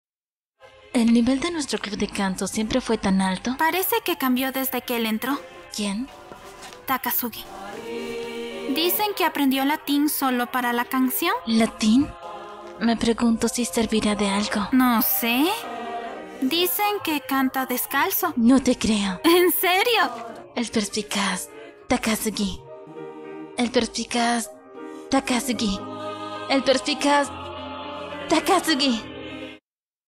Comienzo｜El Perspicaz Takasugi (30s)
¡Hola! En esta ocasión les comparto el 3º episodio de la serie de comerciales de la Empresa de Telefonía Japonesa AU｜El Perspicaz: Takasugi. Espero que sea de su agrado ^^

¡Muchas gracias al talentoso elenco que ha colaborado en este proyecto!